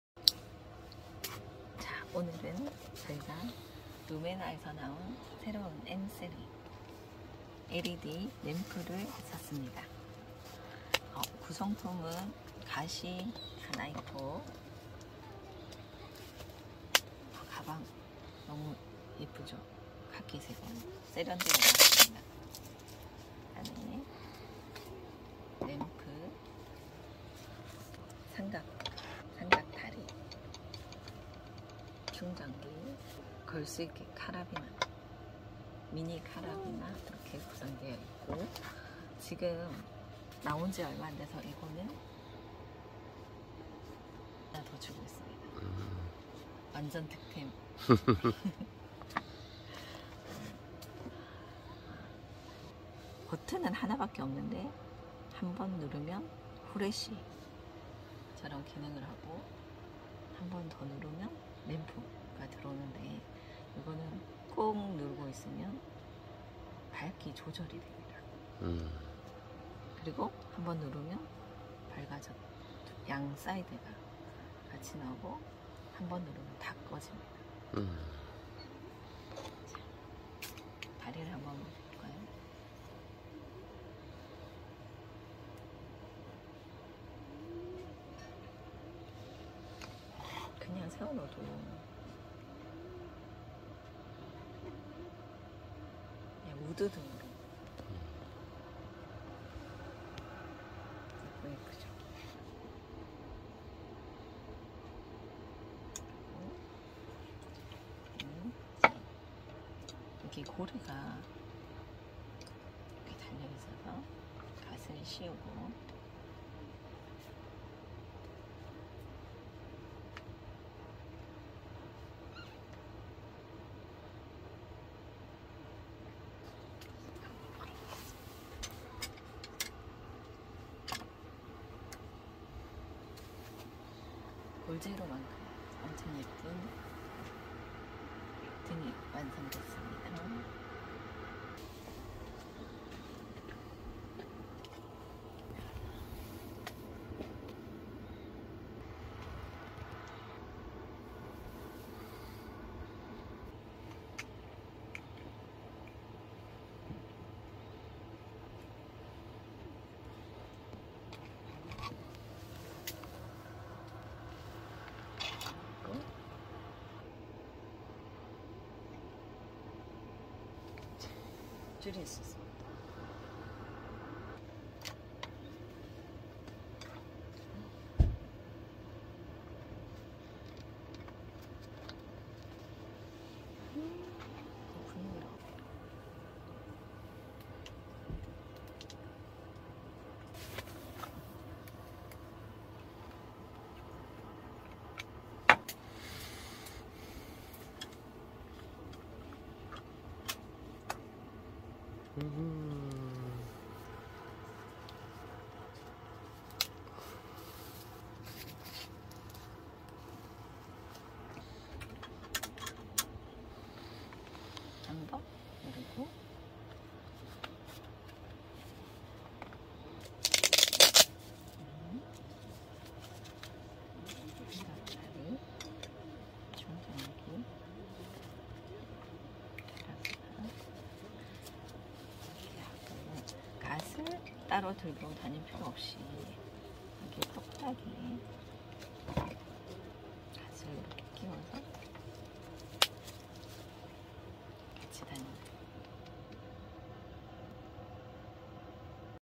루메나M3 멀티플 LED랜턴//감성+기능끝판왕
루메나M3 멀티플 LED랜턴
정품스티커 QR코드 슥캔후 정품등록 무상 AS 받을 수 있엊요
구성
LED랜턴,삼각대 ,C타입 케이블,비너,파우치포함,쉐이드
가격 59000원
집중광/ 확산광 최고등급으 CREE LED  사용 고출력 ,고효율 밝고 은은함
삼각대를 결합시켜 쉐이드를 씌우면 테이블 강성랜턴이 됩니다
자전거 탈때 전조등 및 산행및 낚시할때 손전등으로 사용
밝기 조절 3단계
최대밝기 300루멘
최다사용시간 200시간 베터리 LG화학 정품셀/ 방수 방진 등급 IP67(생활방수기능 갑작스럽게 내리는 비나 눈등에 제품보호)
삭상 2가지 실버와무광베이지
전용파우치 쉐이드홀더에  쉐이드거치